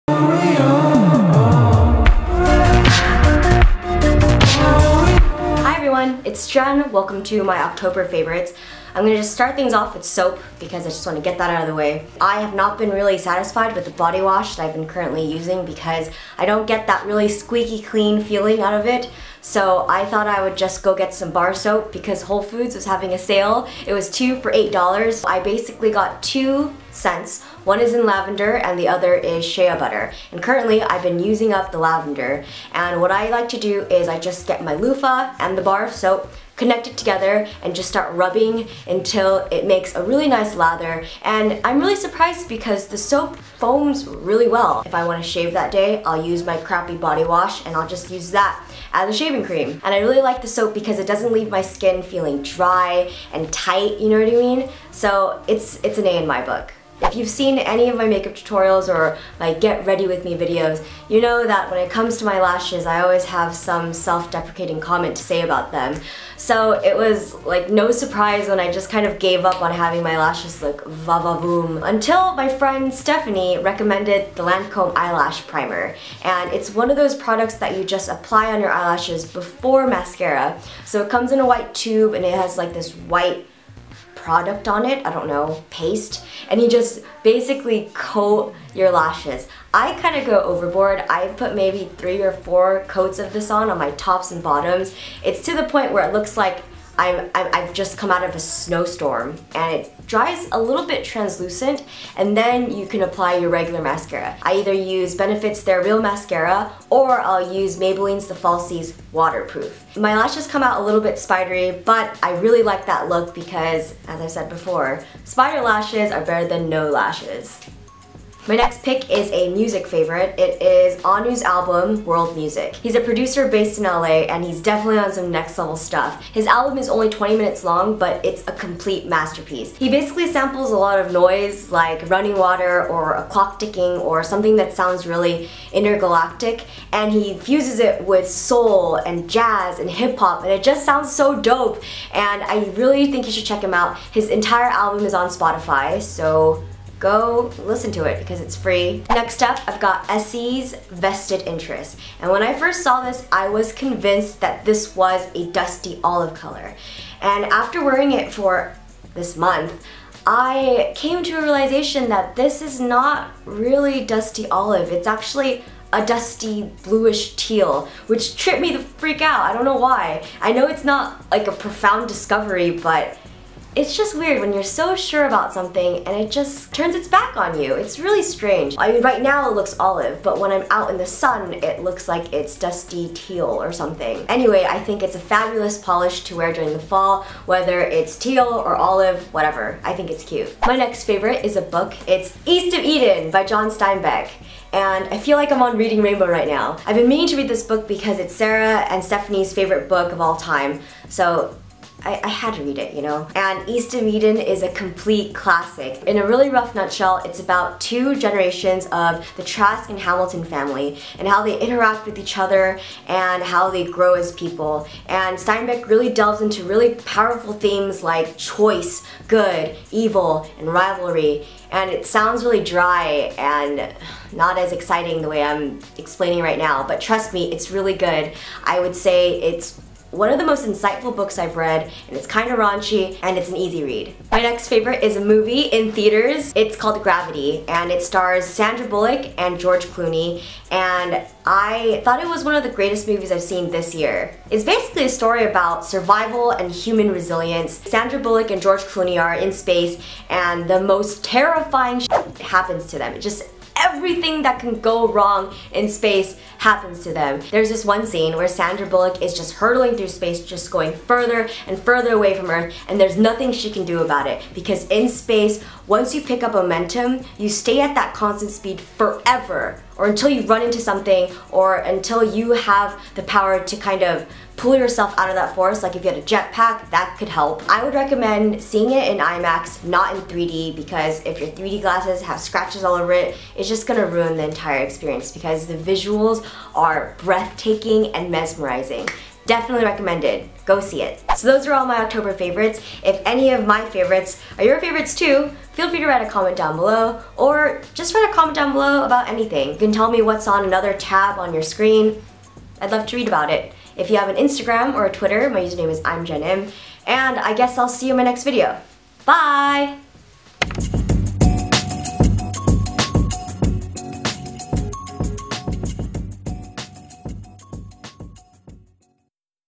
October Favorites 2013
SPEAKING PORTION

- Lips: Make Up Forever Aqua Rouge #17
- Top: Lovers and Friends

THINGS MENTIONED

→ One With Nature Soap: Lavender & Shea Butter

→ East of Eden by John Steinbeck